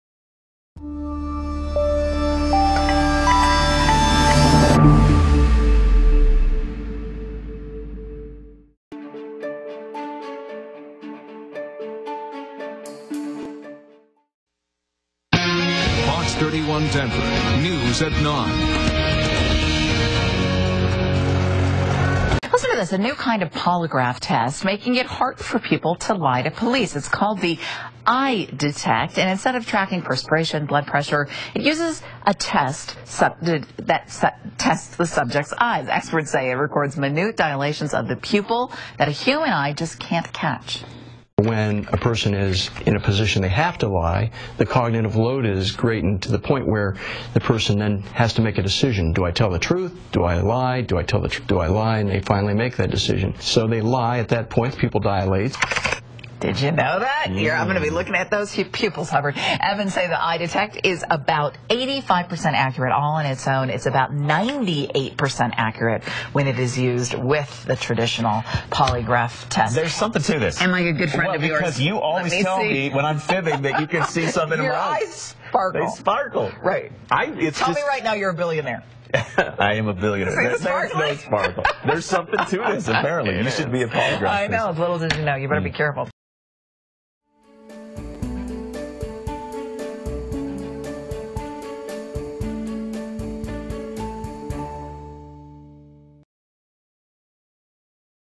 New Polygraph Test Tracks Pupils
In this video, a TV station in Denver, CO reports on this innovative technology.

The main benefits of EyeDetect include:

• High accuracy
• Fast (only 30 minutes for a test)
• Nonintrusive
• Cost-effective
• Portable